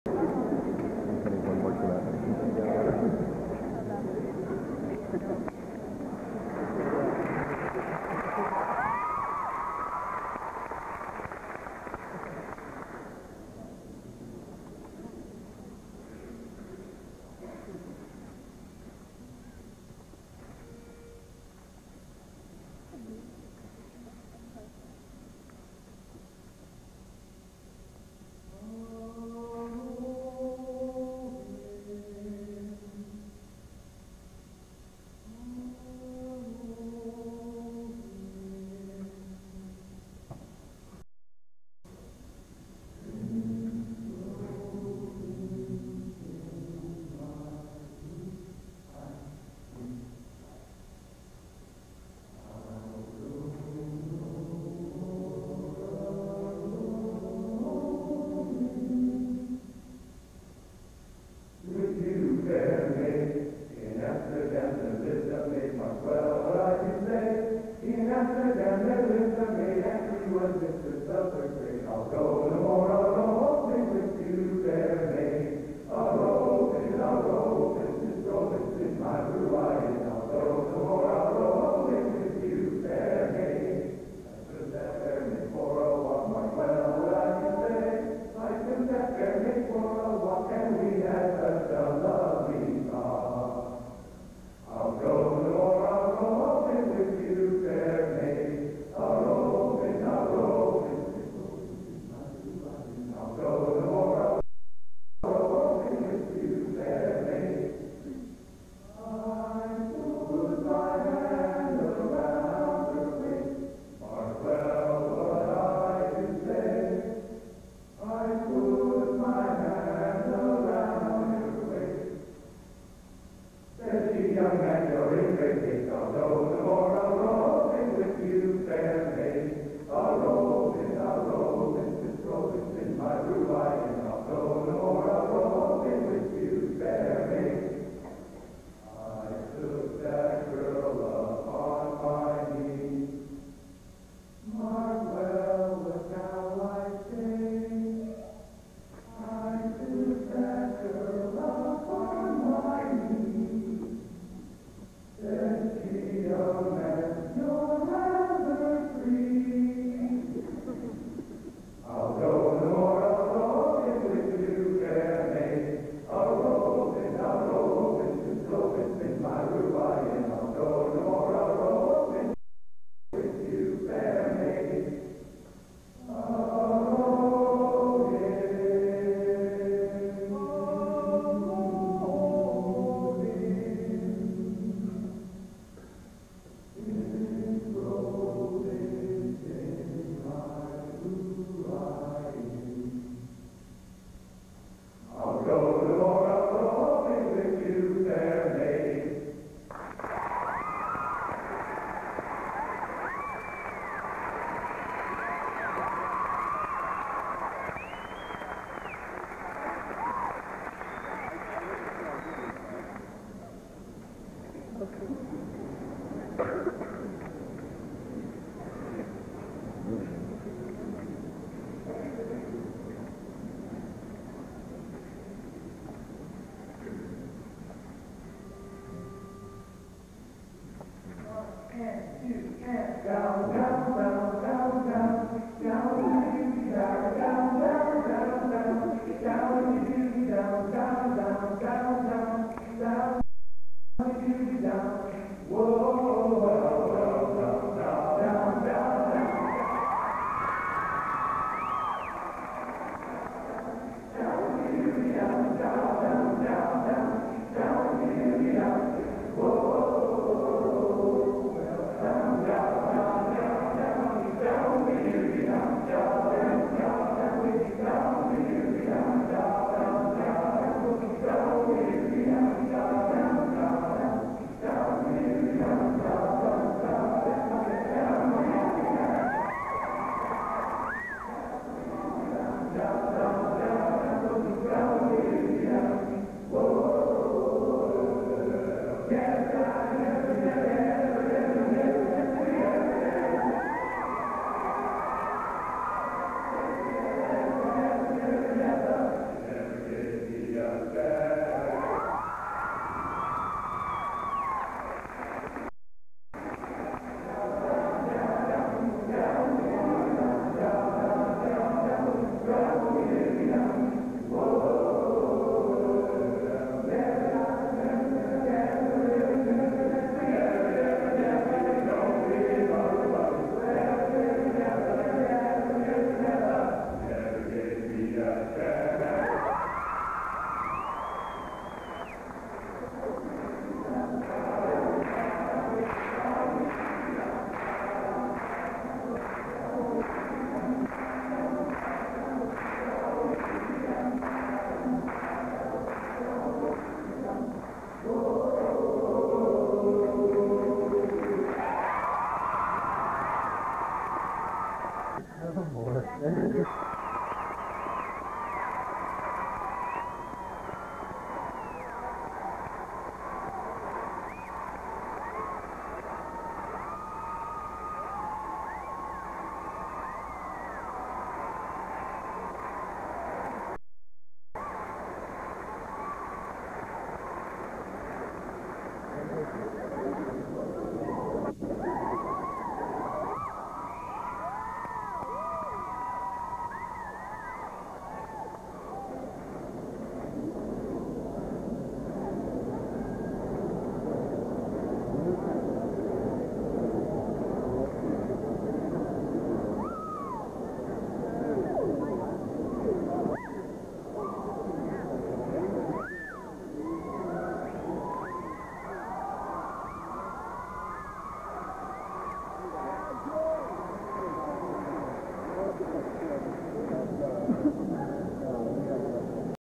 Delta Tau Delta UW Songfest 1989
This is a video of Delta Tau Delta performing in Kane Hall at 1989 University of Washington Greek Week.